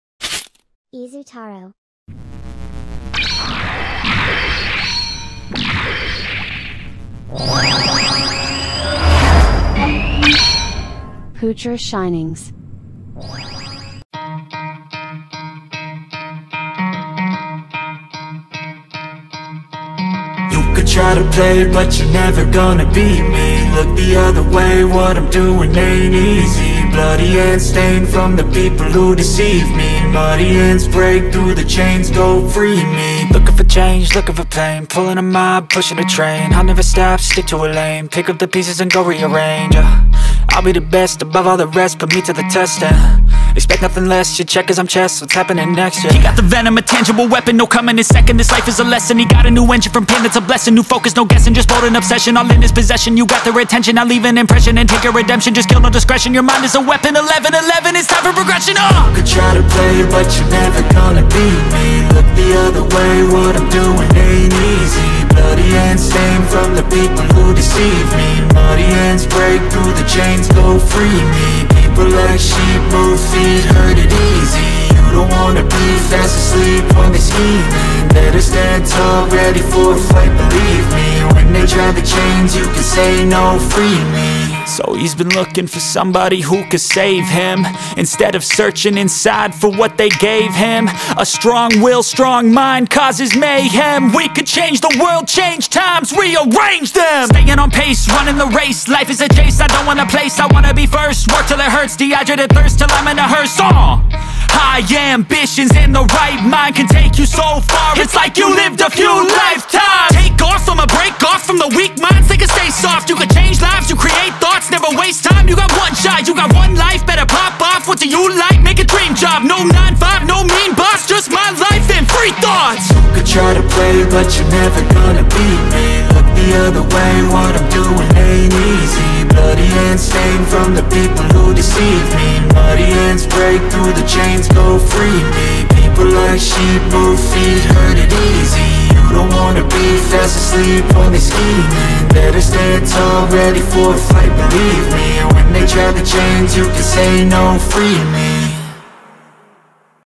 Before You Buy MG GAT-X207 Blitz Gundam Quick Review | GAT-X207 ブリッツガンダム
The GAT-X207 Blitz Gundam is a prototype stealth Mobile Suit introduced in the anime Mobile Suit Gundam SEED. It is piloted by Nicol Amalfi.

If you love Gunpla, follow this one

[Blogger]
All my customize Gunpla and toys